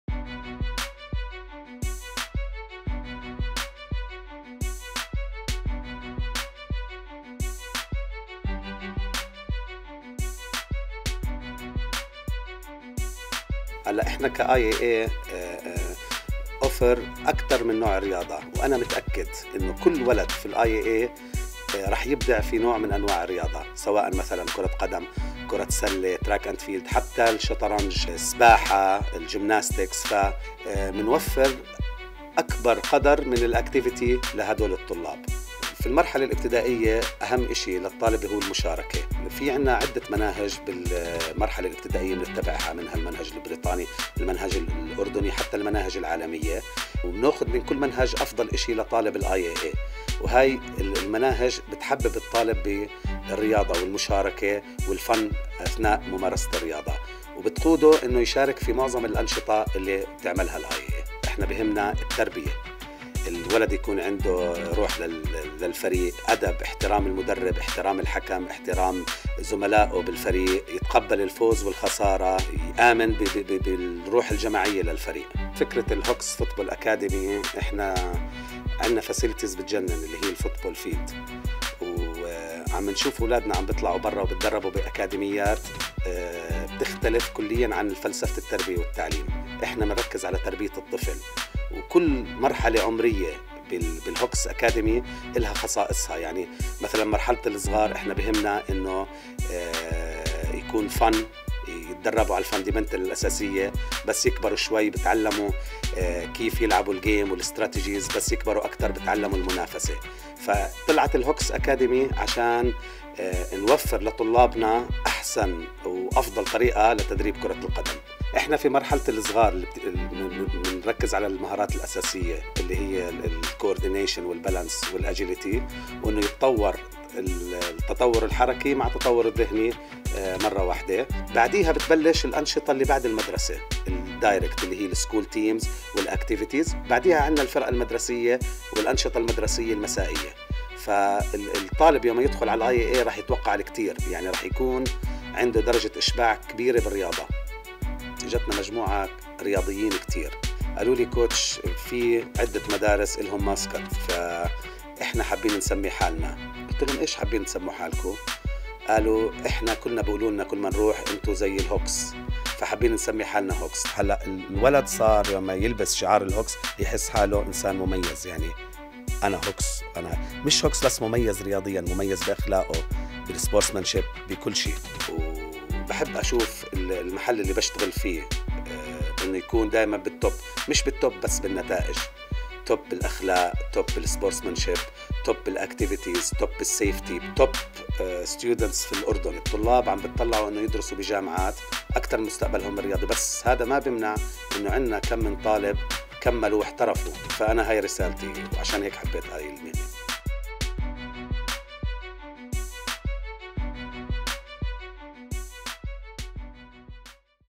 IAA Hawks: Our Proud Athletes
Watch this video to learn about the International Academy - Amman (IAA) Athletic Programme, the IAA Hawks Football Academy and the IAA Hawks athletes. We are the proud IAA Hawks!